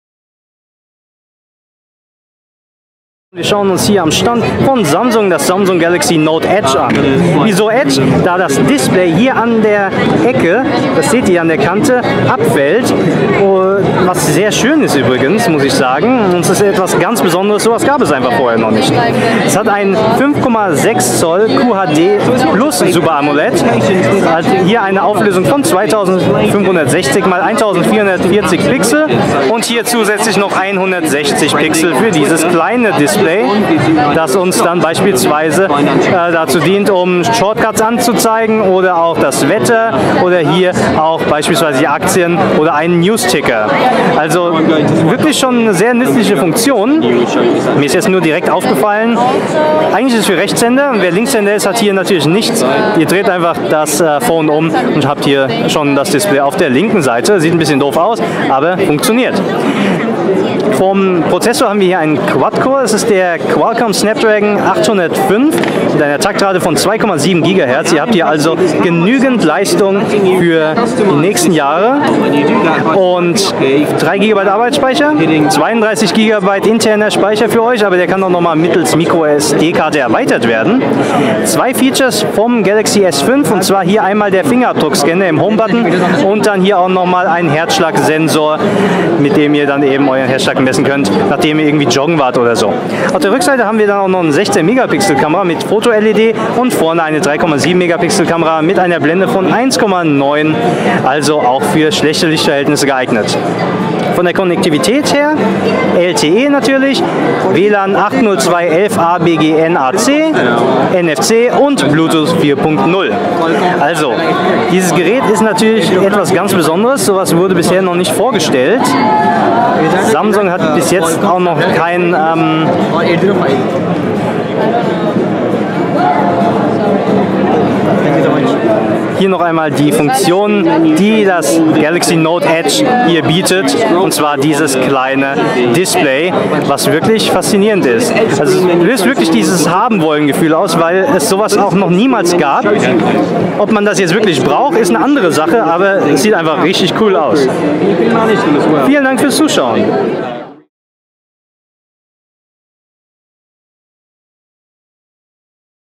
IFA 2014: Samsung Galaxy Note Edge
Ein weiteres Highlight auf der IFA 2014 war das Samsung Galaxy Note Edge mit seinem gebogenen Display am rechten Rand. Dieses dient als Anzeige für Widgets, Newsfeed oder Börsenkurse. Alle weiteren Features erklärt unser Redakteur Johannes im Video.